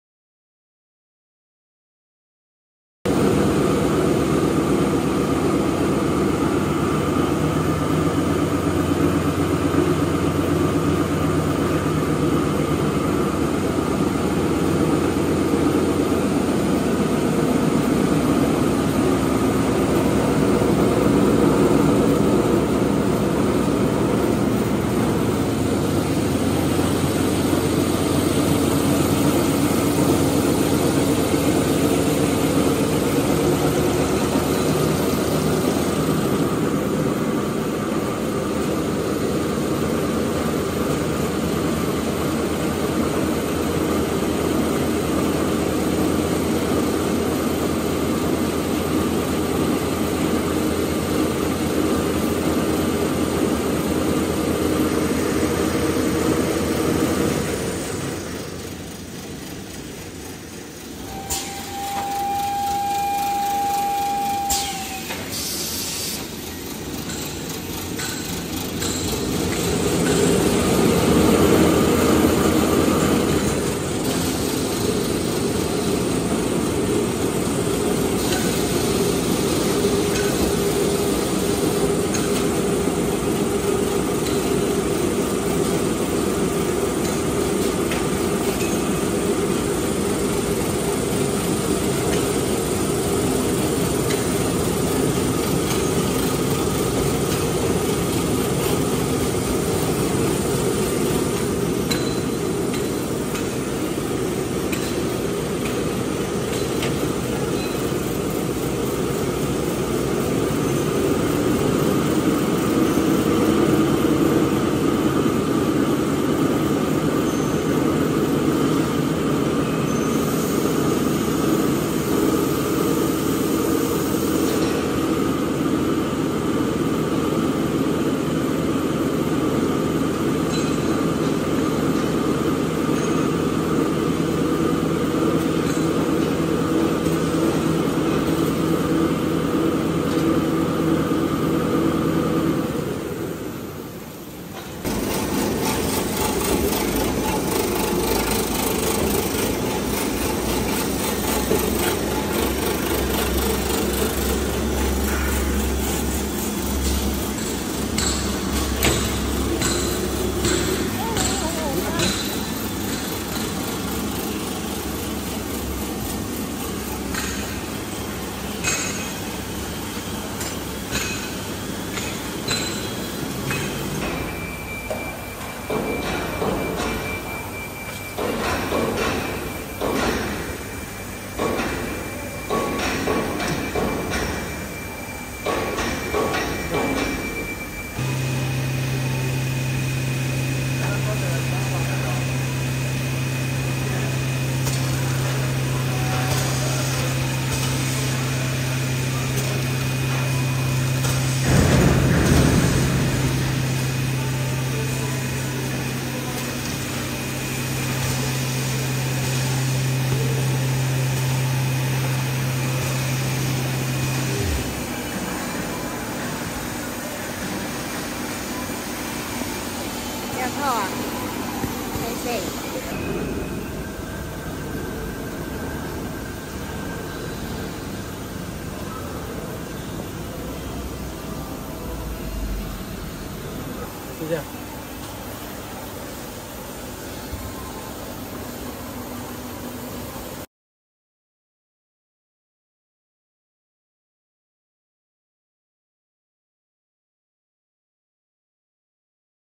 Maxi Roll Tissue Paper Making Machine Production Line Paper Towel Coil Machine
This Maxi Roll Tissue Paper Production Line including 1. Rewinding Machine 2. Cutting Machine 3. Shrink Packaging Machine